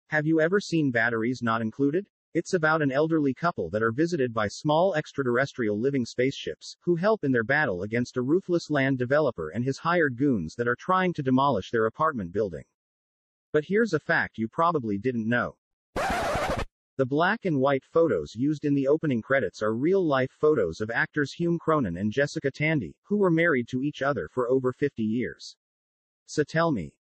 *batteries not included (1987) - Movie Facts
Batteries Not Included (stylized as *batteries not included) is a 1987 American science fiction comedy film directed by Matthew Robbins about small extraterrestrial living spaceships that save an apartment block under threat from property development. The story was originally intended to be featured in the television series Amazing Stories, but executive producer Steven Spielberg liked the idea so much that he decided to adapt it into a film. It was the feature film screenwriting debut of Brad Bird.